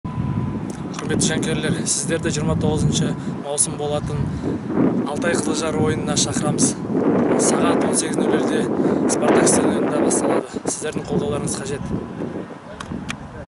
Даурен Кайкибасов "Алтай" - "Кызыл-Жар СК" кездесуіне шақырады
"Алтай" командасының шабуылшысы Даурен Кайкибасов "Кызыл-Жар СК" командасымен кездесуіне шақырады. Ойын 29 маусым күні сағат 18:00 "Спартак" стадионында өтеді.